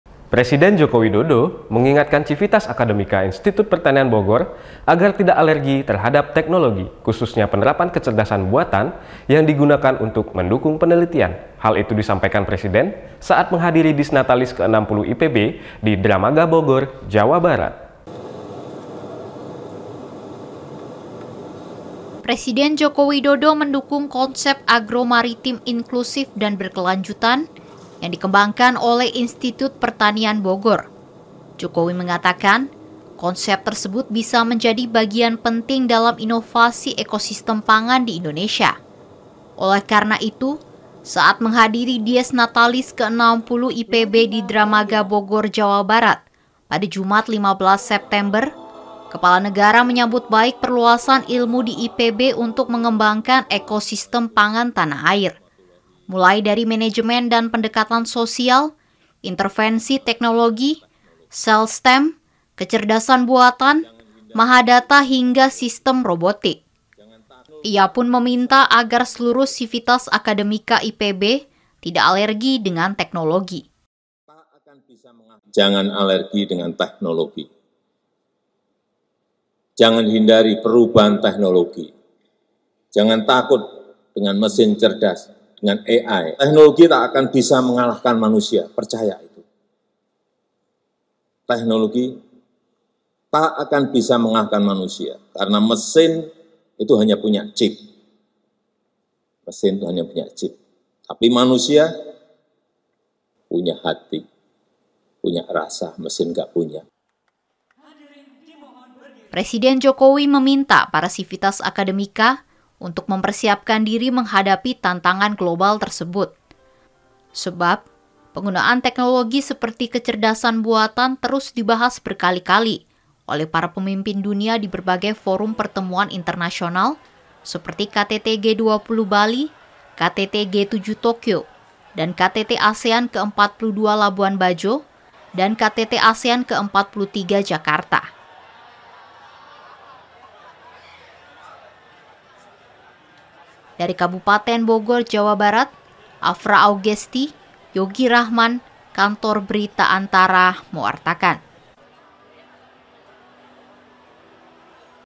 PRESIDEN INGATKAN CIVITAS AKADEMIKA IPB: JANGAN ALERGI TEKNOLOGI || BERANDA NUSANTARA
Presiden Joko Widodo mengingatkan civitas akademika Institut Pertanian Bogor (IPB) agar tidak alergi terhadap teknologi, khususnya penerapan kecerdasan buatan (AI), yang digunakan untuk mendukung penelitian. Hal itu disampaikan Presiden saat menghadiri Dies Natalis ke-60 IPB di Dramaga, Bogor, Jawa Barat